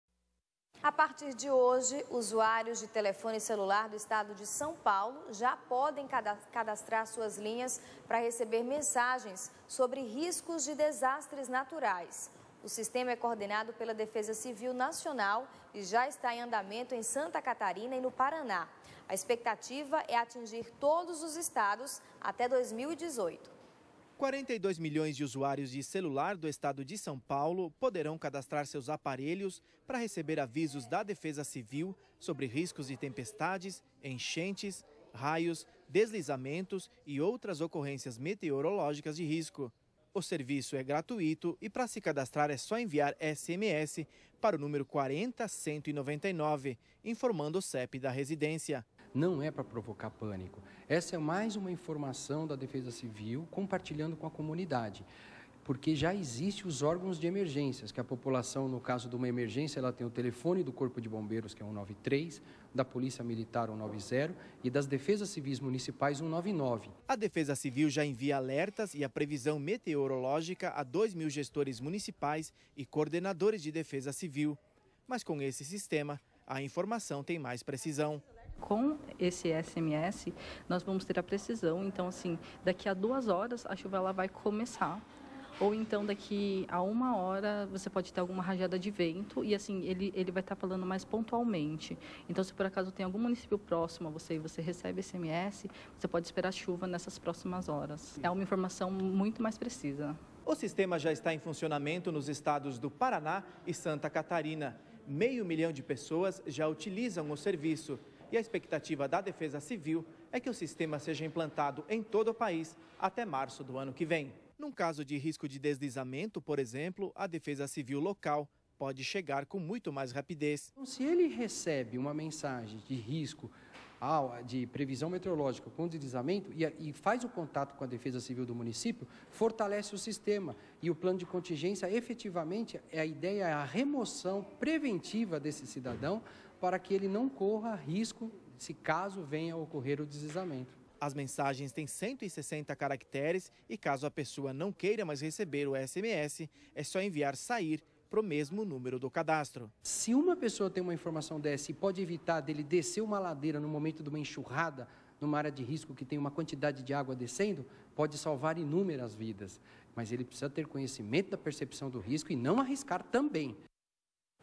São Paulo passa a ter serviço de alerta para desastres naturais-CN Notícias
Agora usuários de telefone celular do estado de São Paulo já podem cadastrar suas linhas para receber mensagens sobre riscos de desastres naturais. O sistema é coordenado pela Defesa Civil Nacional e já está em andamento em Santa Catarina e no Paraná. A expectativa é atingir todos os estados até 2018.

Reportagem de Sidinei Fernandes e Rodrigo Tedesco